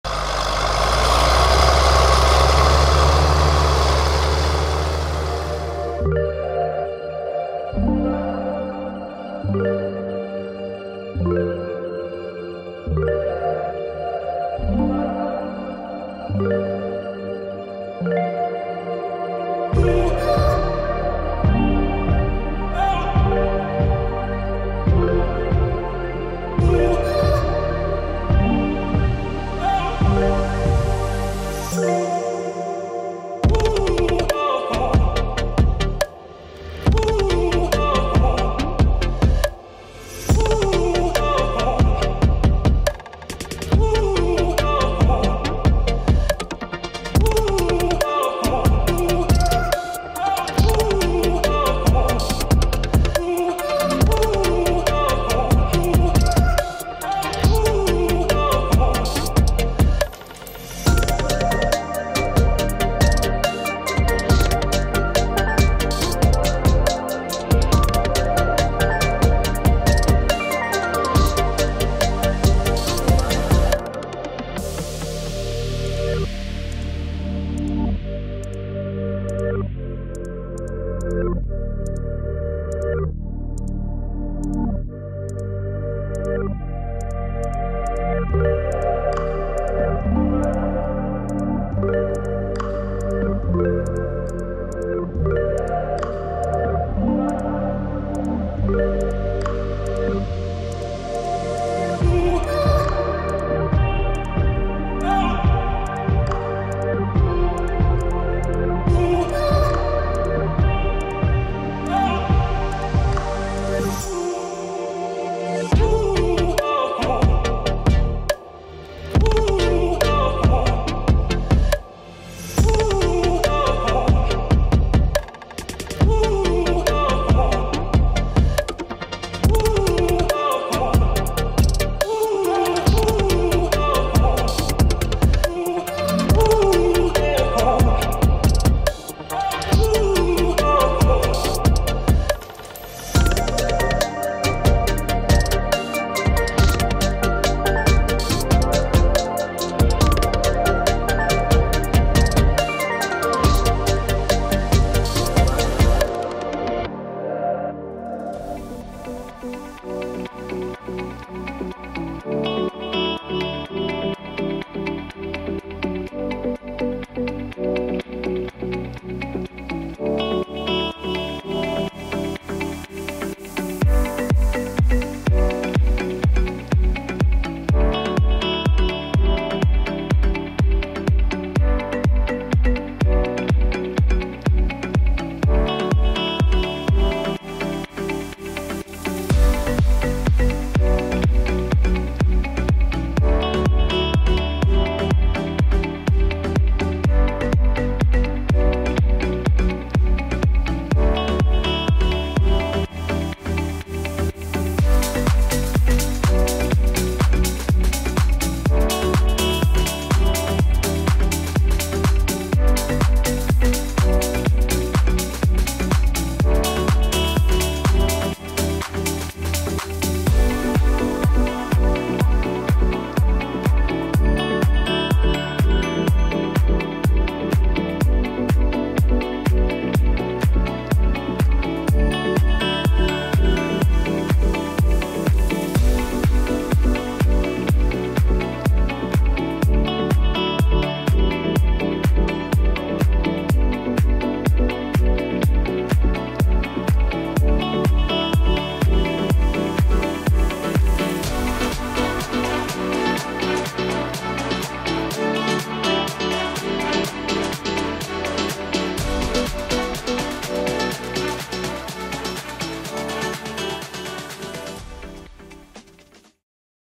Was wir dieses Jahr alles unterm Hintern hatten🤯💪 | 7810 Diaries & Bauer Struck!
Dieses Jahr war schon krass! Vielen Dank an EUCH, für Zuschauen, Kommentieren und Unterstützen, auf allen Formaten. Nur dadurch konnten wir so tolle Maschine zeigen und nach jetzigem Stand wird es 2024 mindestens genauso verrückt:) Einen guten Start ins neue Jahr, wir sehen und hören uns!